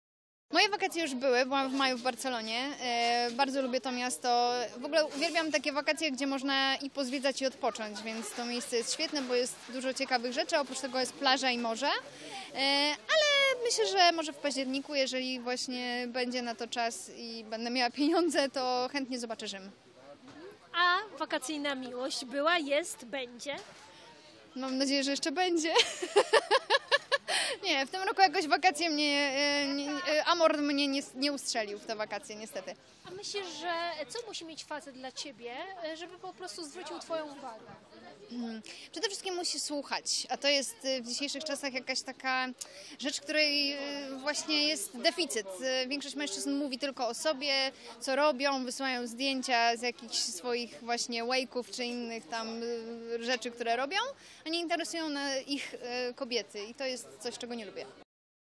Joanna Derengowska: niestety Amor nie ustrzelił mnie w te wakacje
Aktorka ma nadzieję, że podczas tegorocznego lata spotka ją jeszcze romantyczna przygoda. Jak dotąd bowiem nie przeżyła wakacyjnej miłości. Gwiazda twierdzi, że cechą, na którą szczególnie zwraca uwagę u mężczyzn, jest umiejętność słuchania i zainteresowanie drugą osobą.